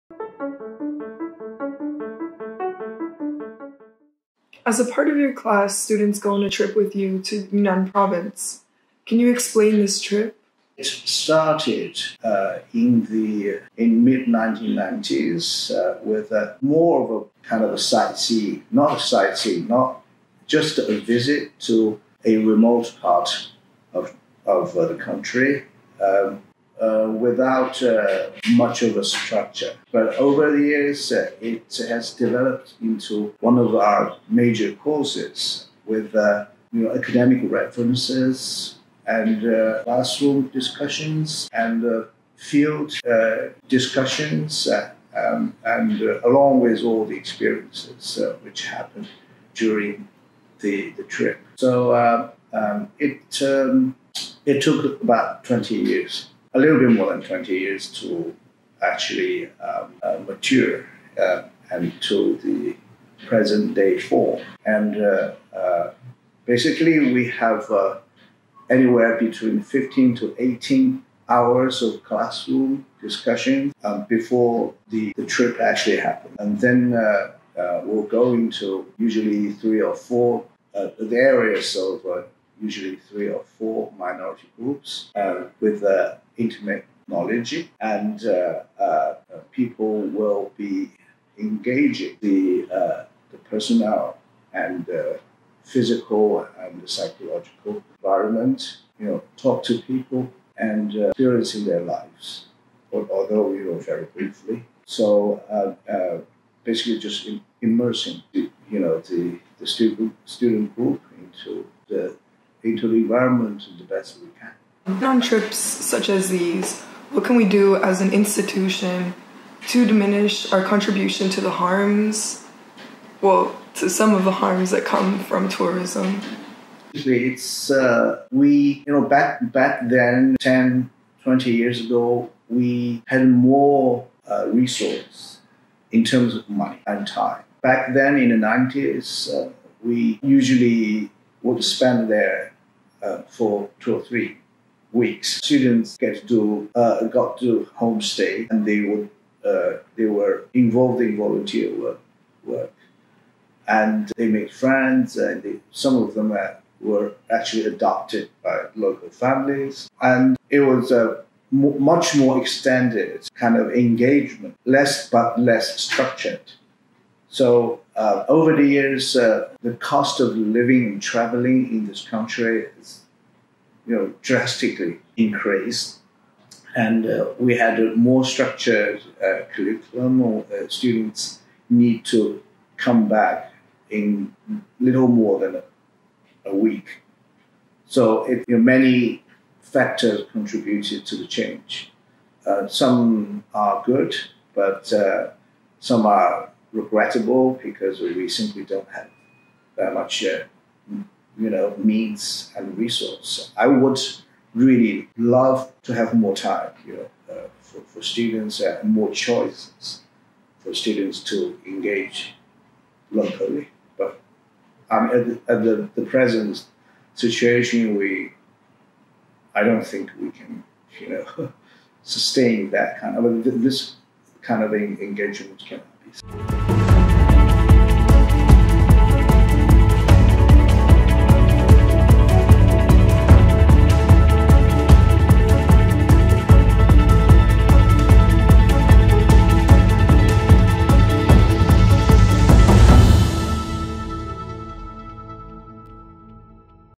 The Yunnan Trip feat Professor Liu Wei│ LIU Global CHINA
Every Spring students have the opportunity to travel to culturally diverse Yunnan, the western province adjacent to Vietnam, Laos, and Myanmar. Regina Olivares talks to Professor. Liu Wei about this trip that has been an important part of the curriculum since the 90s.